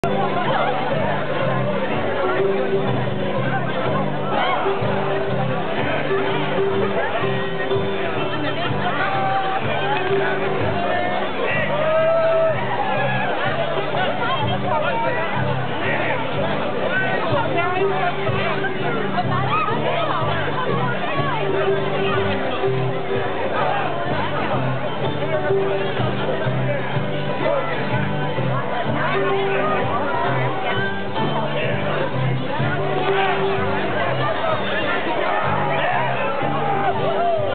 ROYAL COURT DANCES AT OPENING OF TRF 2011
As part of the Opening Gate Ceremonies at TRF, the Royal Court engages in several colorful dances specially designed to get visiting patrons into a festive mood for the day. It's also one of the few times during the faire day when patrons can actually get to see most of the cast and characters all together at the same time, simultaneously! Check out ryleh.org for more RenFaire videos and pics at your convenience